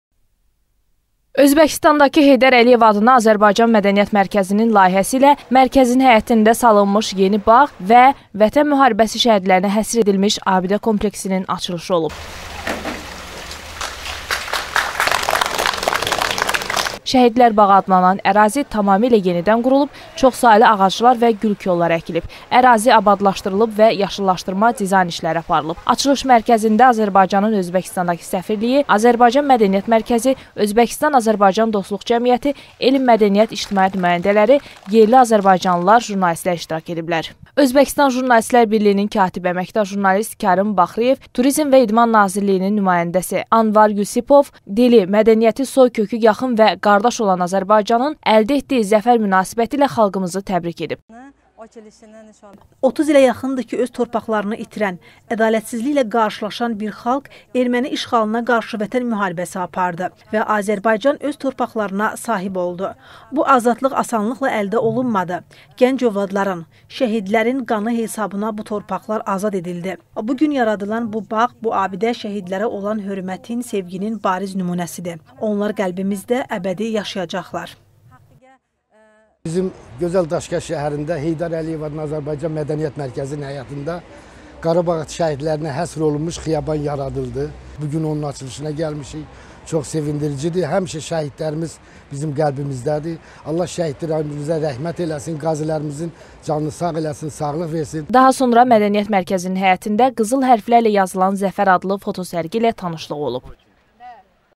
Özbəkistanda “Şəhidlər bağı”
Vətən müharibəsi şəhidlərinə həsr edilmiş kompleksin açılışı olub